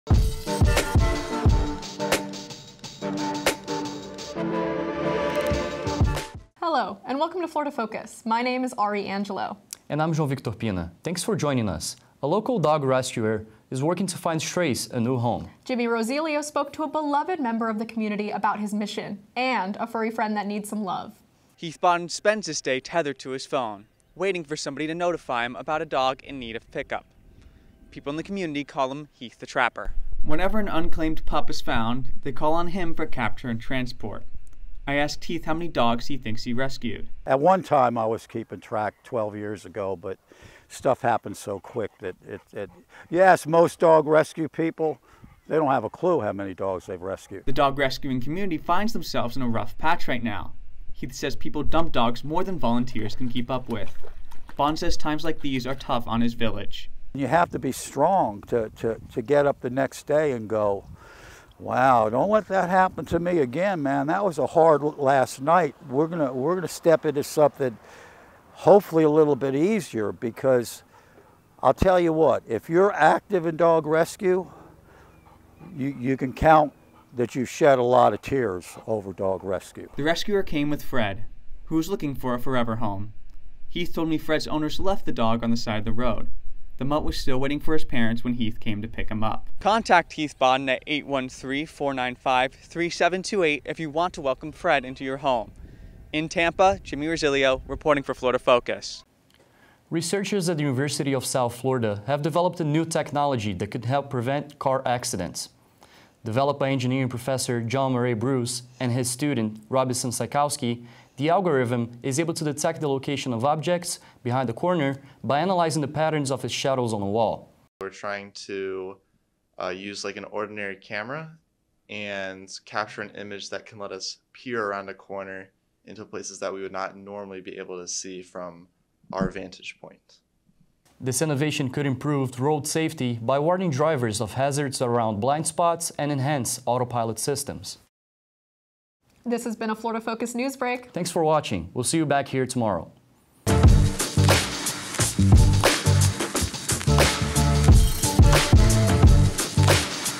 03/07/24 Local hero finds homes for stray dogs
Heath Bodden started rescuing homeless dogs over a decade ago and is now known for serving his community.

When asked how many dogs he’s saved, Bodden said “50, 60 I don’t know, I’ve really lost track. At one
time I was keeping track, but stuff happens so quick.”

“You ask most dog rescue people, they don’t have a clue how many dogs they’ve recused.”

The community refers to him as “Heath the Trapper” as a play on his mission. He waits by his phone for calls about abandoned pets and adoption inquiries.

Fred (shown below) is one of the dogs looking for a new home.

He did not get adopted during his stay at the Hillsborough Pet Resource Center. Bodden picked him back up when he was told that Fred would be put down.

New car safety technology from USF
USF researchers developed a new technology that could prevent car accidents.

Professor John Murray-Bruce and his students made an algorithm that finds the location of objects around corners. The program analyzes shadows cast on surfaces to estimate the source’s position.

“We’re trying to use like an ordinary camera and capture an image that can let us peer around the corner into places that we would not normally be able to see from our vantage point,” explains graduate student Robinson Czajkowski.

The tech defends from accidents caused by blind spots.

Credits
Anchors: Ari Angelo & Joao Victor Ouriques De Souza Pina
Producer: Melissa Baldelomar
Reporters: Jimmy Rosilio & Joao Victor Ouriques De Souza Pina
Graphics: Samantha Allen
Technical Director: Justin Michael
Teleprompter: Samantha Allen
Video Editor: Aviva Kram
Cameras: Victoria Harris
Audio: Abby Papasodero
Web Editors: Ari Angelo & Josh Skinner
Graduate Assistant: McKenzie Muskett
Faculty Advisor: Dr. Stephanie Anderson